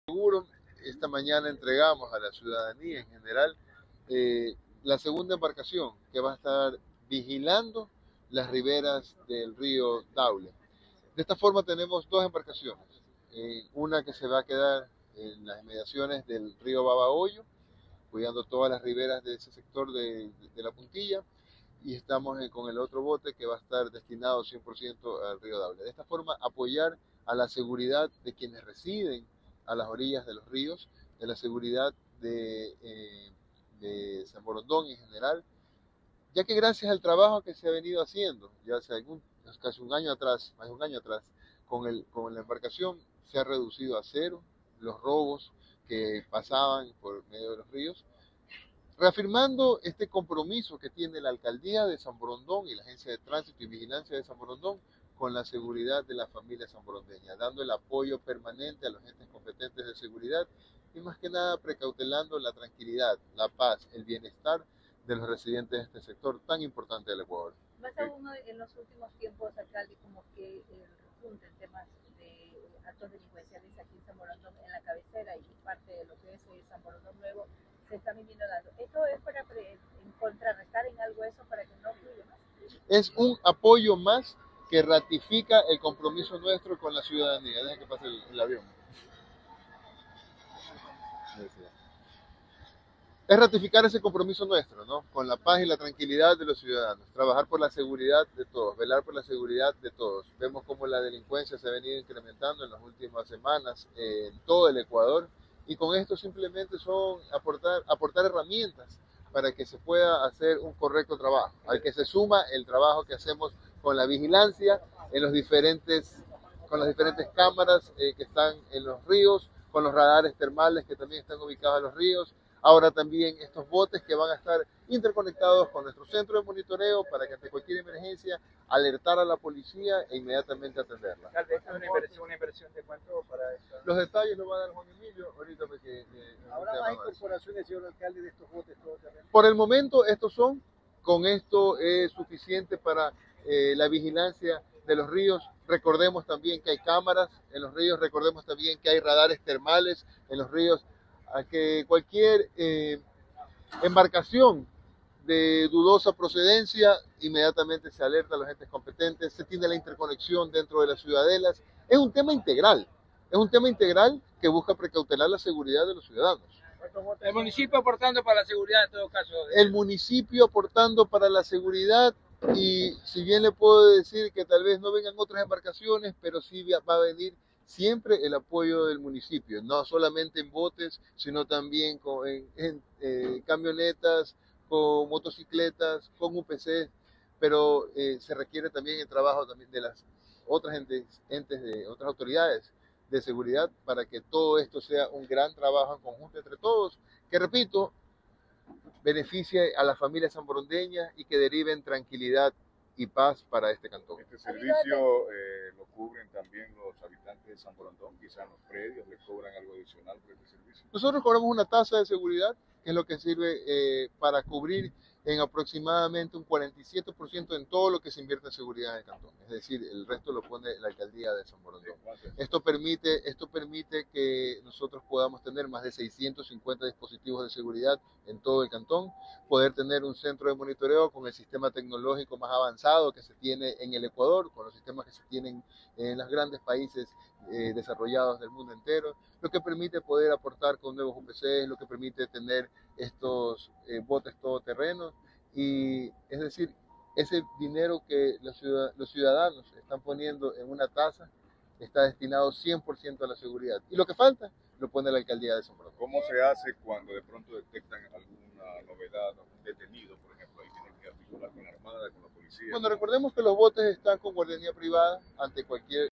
Se suma un segundo bote todoterreno para el patrullaje fluvial en Samborondón
La Alcaldía de Samborondón, encabezada por el ingeniero Juan José Yúnez, puso en función esta mañana, a favor del cantón, su segundo bote todoterreno con el objetivo de vigilar la zona fluvial y así evitar el ingreso de vehículos marítimos que no cumplan las normas de control establecidas
Con el primer bote todoterreno se lograron atender 300 casos de embarcaciones de dudosa procedencia para alertar a la policía y así precautelar la tranquilidad y paz de los samborondeños; hoy, como parte del plan, se adquirió un segundo bote. Esta inversión es de 350 000 dólares al año que comprende combustible marineros y mantenimiento de los vehículos.
Esta compra permitirá implementar el patrullaje fluvial y reforzar la vigilancia nocturna en los ríos que bordean el cantón Samborondón: Daule (Desde las riberas de la urbanización Matices hasta el extremo sur de la ciudadela La Puntilla) y Babahoyo (Desde el norte de la isla Mocolí hasta el extremo sur de la ciudadela La Puntilla) de 18h00 a 06h00.
“El Centro de Monitoreo y Vigilancia de Samborondón desde el 1 de enero hasta el 30 de junio del 2024 ha atendido más de 4700 casos entre activaciones de interconexión de garitas, personas en actitud sospechosa, accidentes de tránsito, alarmas comunitarias, entre otros y desde que implementamos el primer bote todoterreno, no se ha registrado ningún tipo de incidente en el río. Esto evidencia el apoyo a la seguridad del cantón que provee esta alcaldía” mencionó el alcalde de Samborondón, Juan José Yúnez.
Estas dos embarcaciones se suman al patrullaje que realizan el Cuerpo de Bomberos de Samborondón y las Fuerzas Armadas del Ecuador y tienen conexión directa con el Centro de Monitoreo y Vigilancia de Samborondón. Los botes podrán navegar sobre dos pulgadas de agua, pasando por lechuguines, palos, troncos y más elementos flotantes que se presentan en las orillas de los sectores a vigilar.